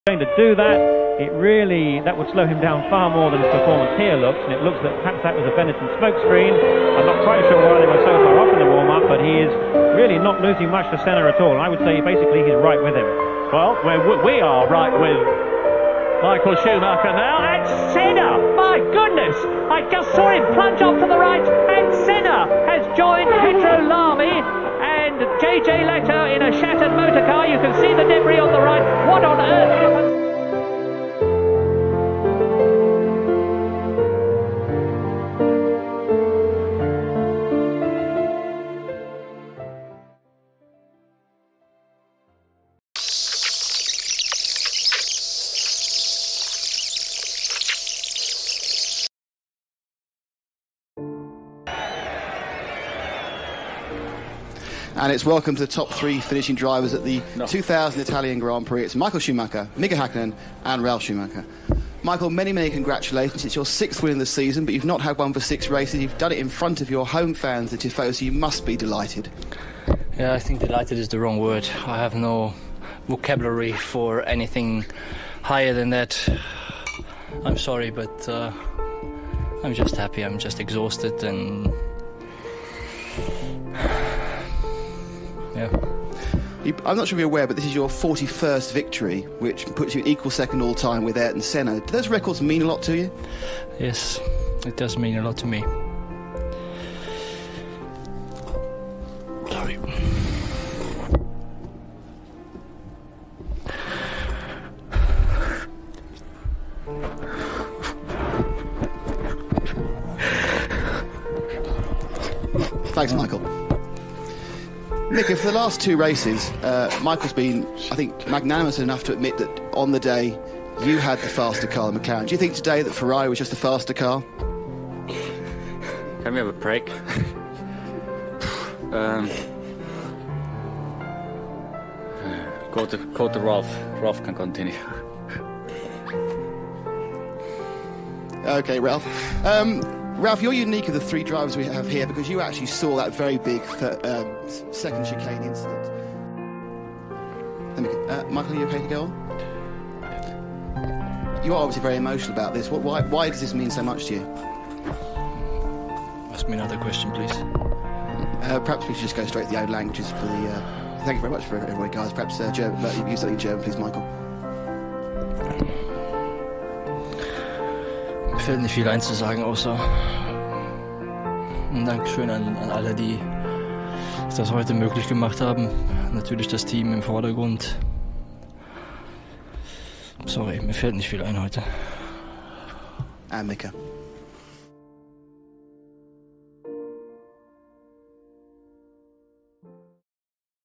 MICHAEL SCHUMACHER | Gets emotional as he equals Senna's record
When Schumacher is asked if it means a lot to him that he equals Senna's records (41 race wins) he starts to cry. The interview is passed on to Mika Hakkinnen, but he cannot answer the question properly due to grief.